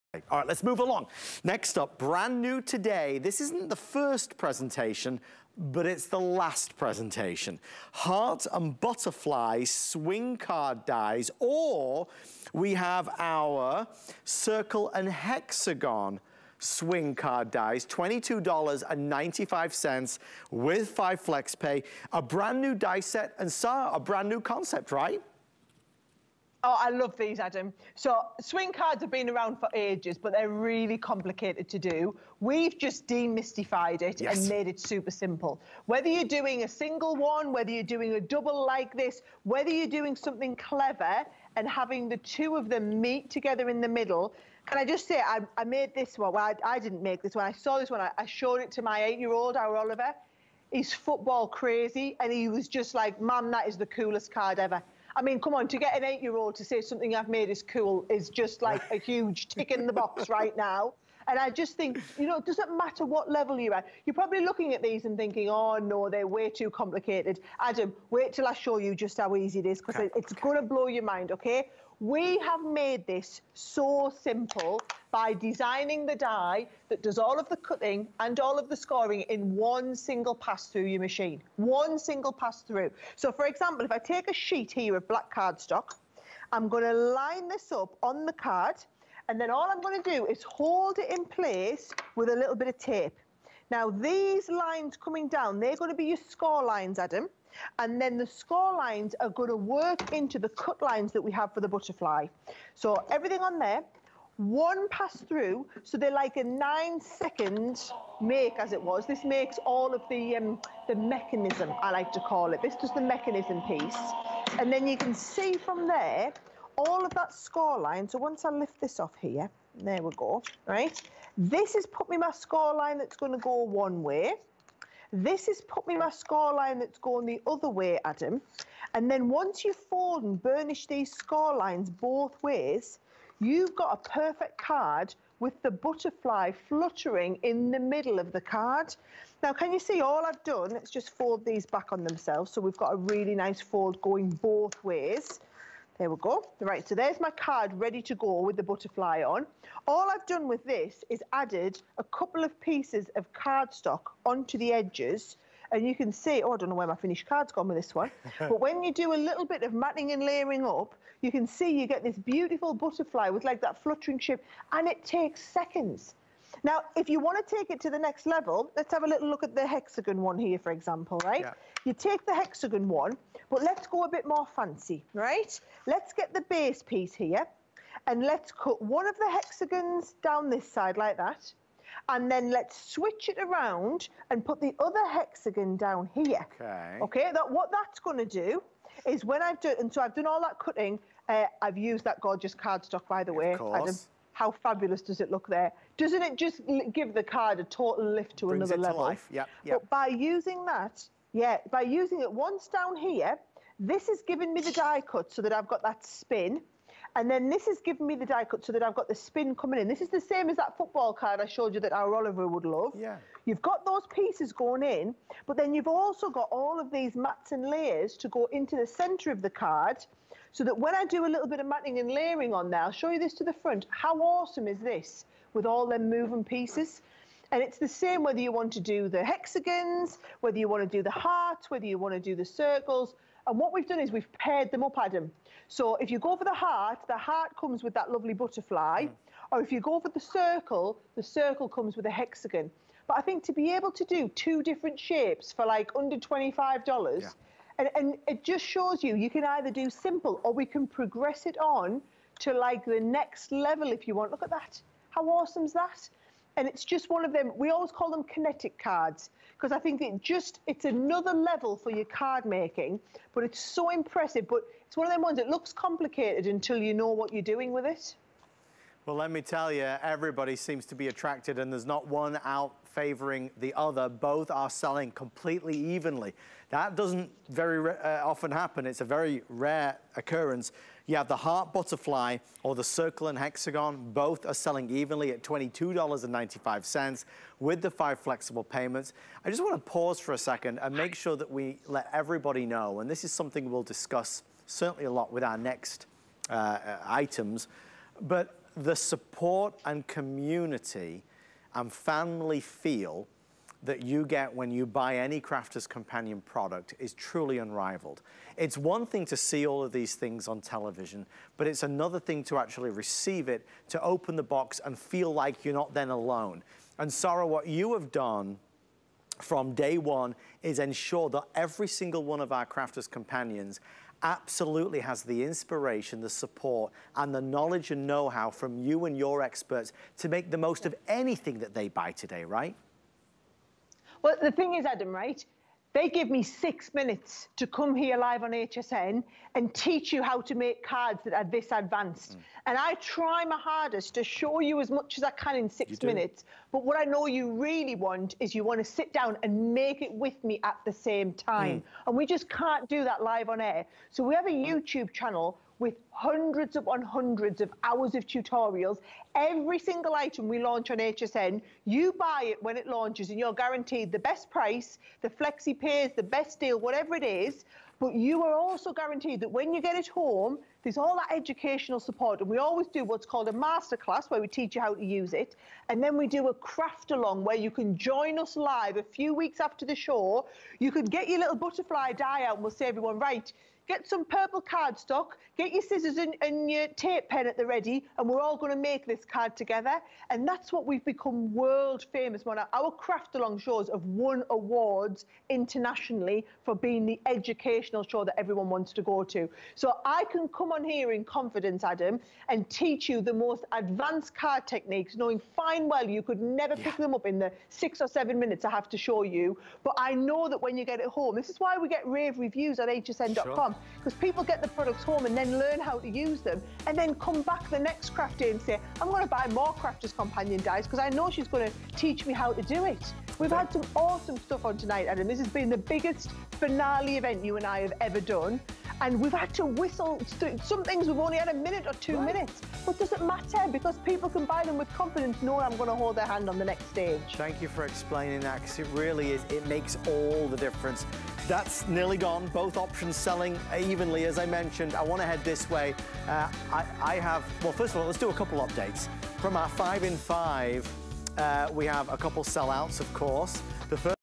Crafter's Companion Heart and Butterfly Swing Card Dies
Create kinetic cards, with ease, using this set of swing card dies. Decorate the designs and add dimension using the additional layering dies (included in this set). It's perfect for all occasions.

    4piece Gemini Metal Die Element Swing Card  Butterfly, approx. 3.9" x 6.2"
    4piece Gemini Metal Die Element Swing Card  Heart, approx. 3.9" x 6.1"

    Some dies may have sharp points, edges or corners.
    Exhibit caution when using these dies and do not leave them unattended near children.
    Not intended for use by any child under the age of 14.